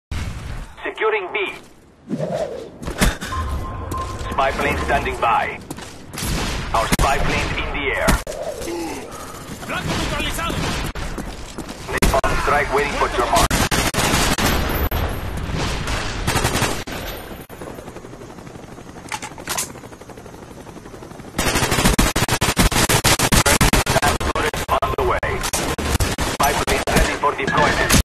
Nice Plays in Havana!
Two plays I got in havana which I think they are pretty nice.